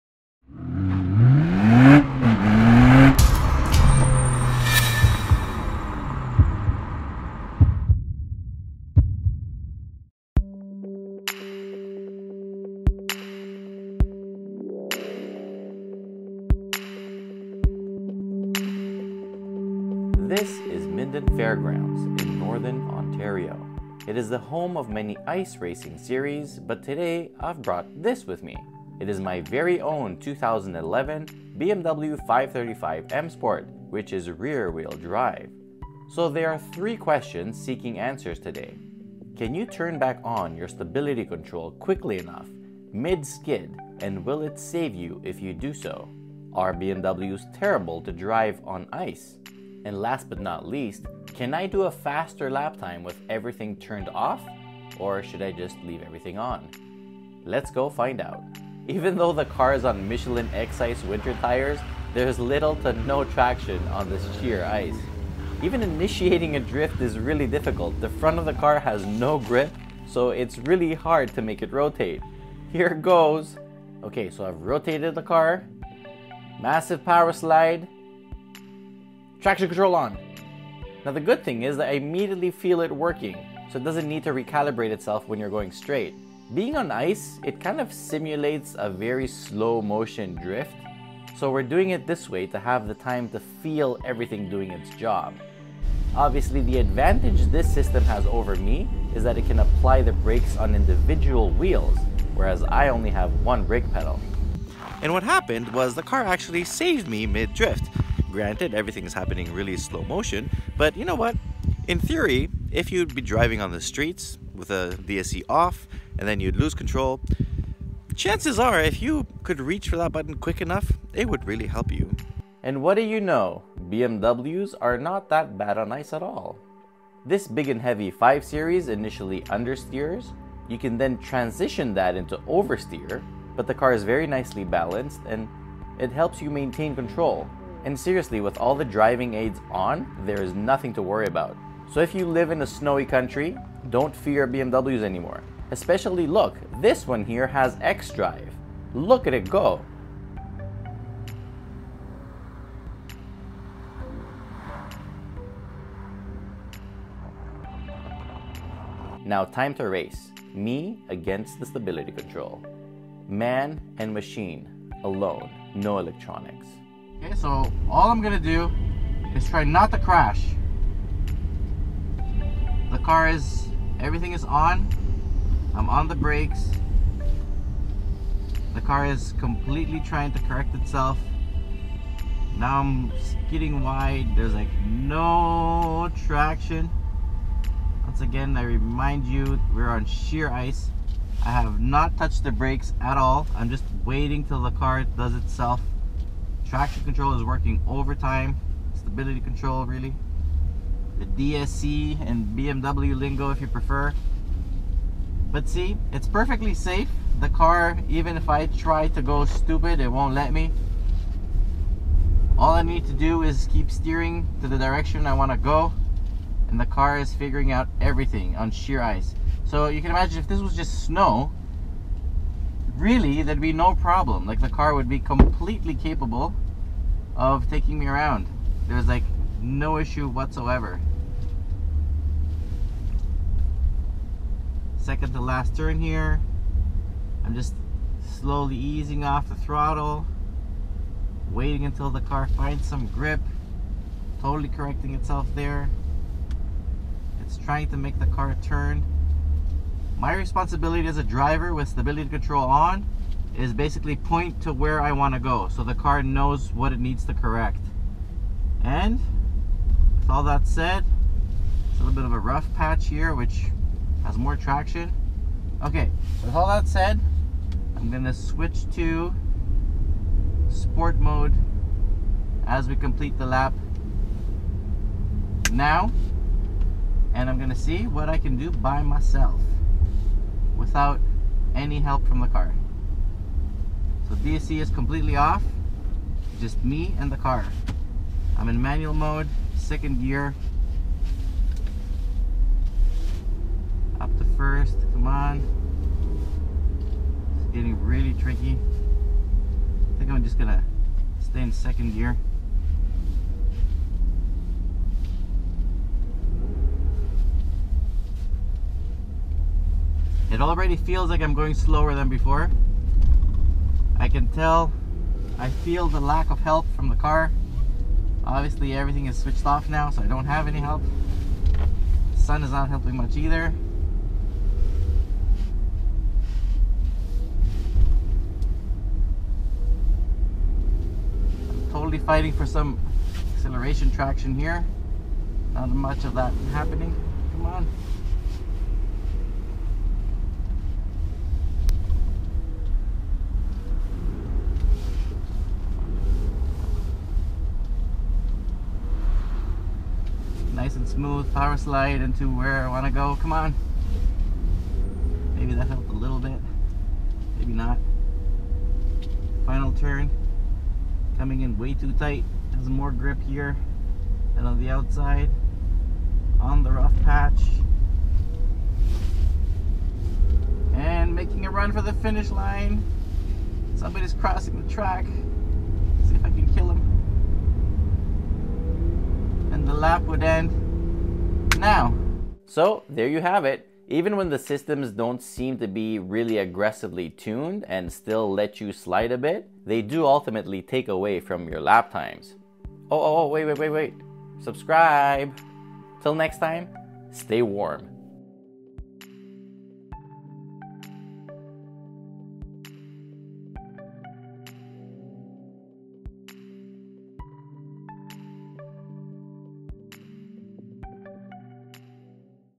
Are BMWs Terrible On Ice? Can ESP Save You Mid-Drift? | Lap Time Comparison | ICEKHANA
Can you re-enable the stability control quickly enough - mid drift - and will it save you if you do so?
Are BMWs just terrible to drive on ice?
Can I drive faster with DSC OFF?